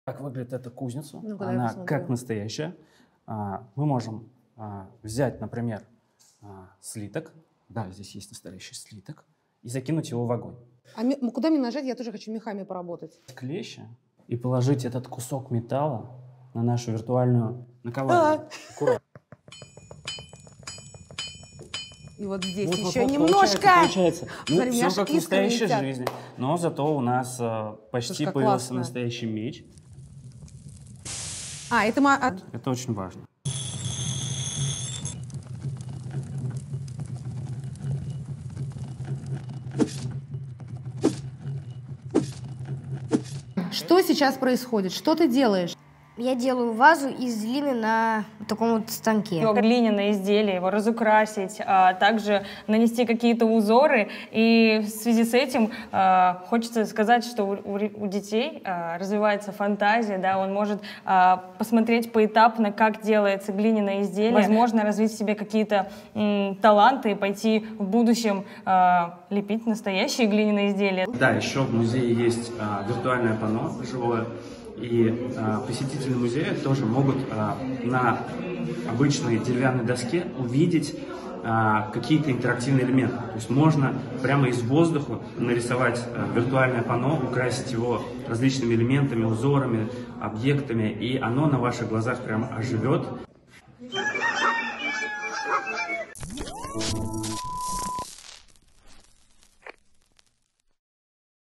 AR для музеев
Соединения реальных и виртуальных экспонатов создаст более глубокий эмоциональный опыт от посещения музея